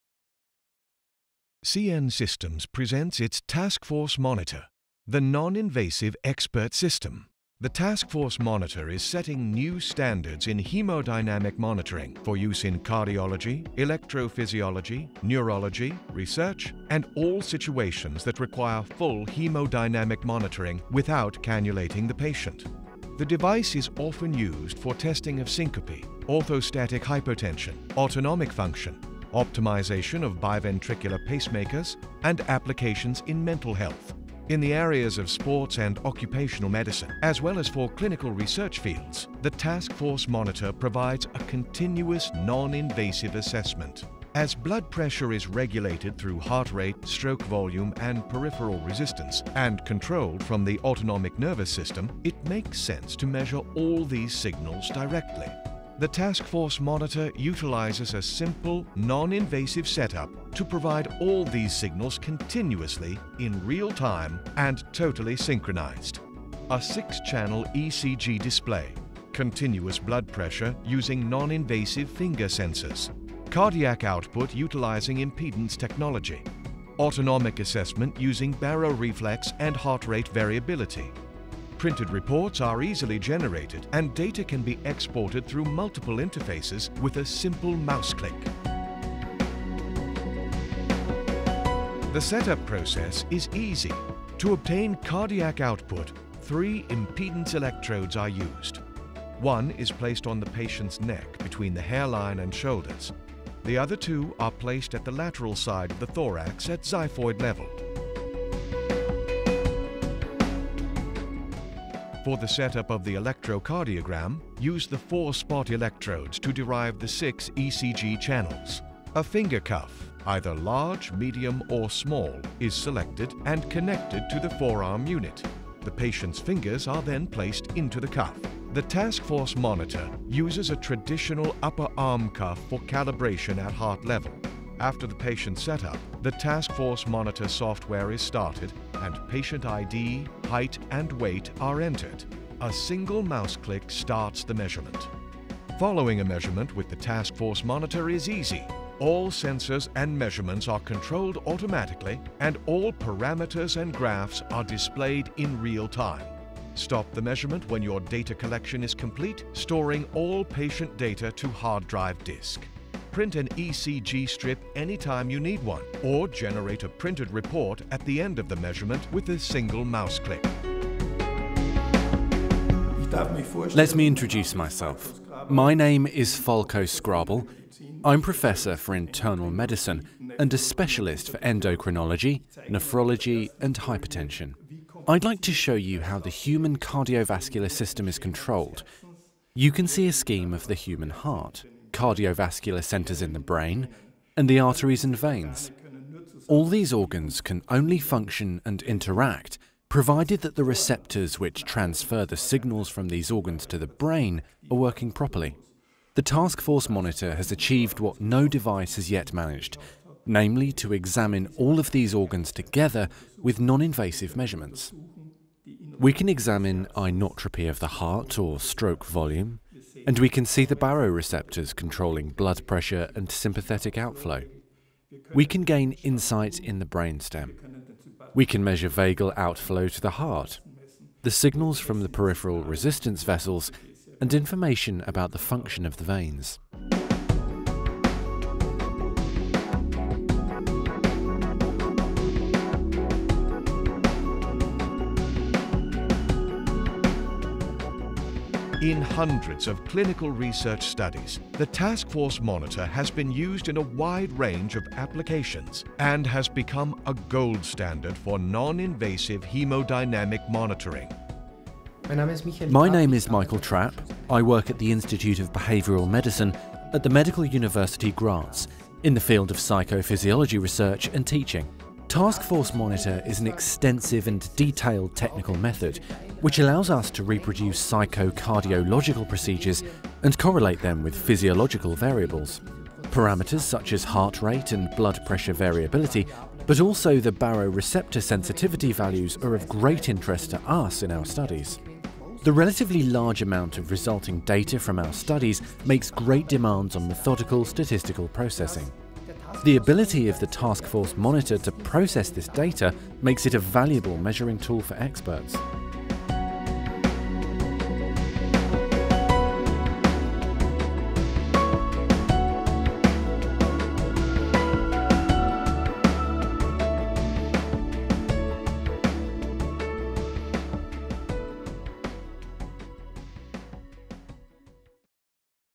Task Force Monitor by CNSystems (full version)
State-of-the art: non-invasive, continuous hemodynamic diagnosis system for experts in research, syncope and autonomic function testing, sports and health science, cardiology, physiology, mental health, etc.